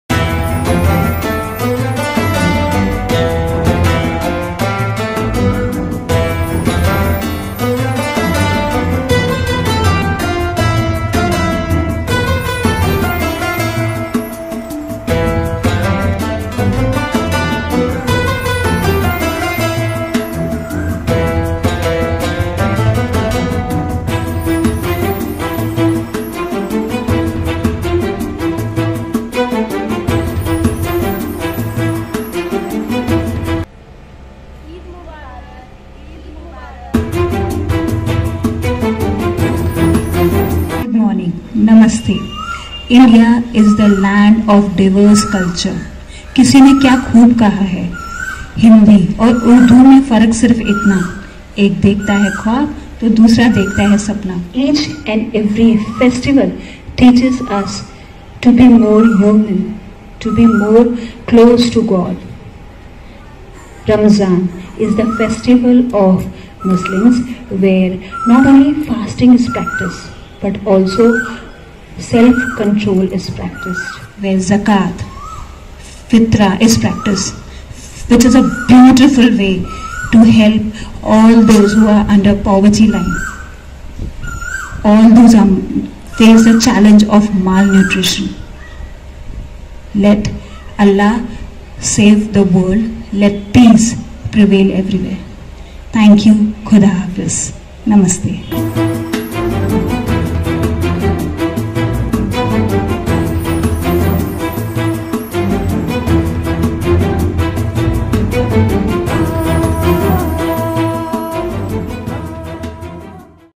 TSSS Team wishes all a Blessed Eid Al-Fitr 2022 - ZajakAllahuKhairan
On the occasion of "पाक  रमजान", Let's follow the teachings of Prophet Muhammad, with the deep understanding of all the ethereal concepts that are relevant to each and every practice during Ramadaan and Eid.

Let's practice "ज़कात" and “सदका", and stand with all those who are underprivileged. Let's fight against malnutrition.

As rightly said by Mother Teresa,

"If you can't feed a hundred people, then just feed one.”

This Eid-al-Fitr, TSSS Family wishes all the beings to come together and experience the joy of giving.

Eid Mubrak to all..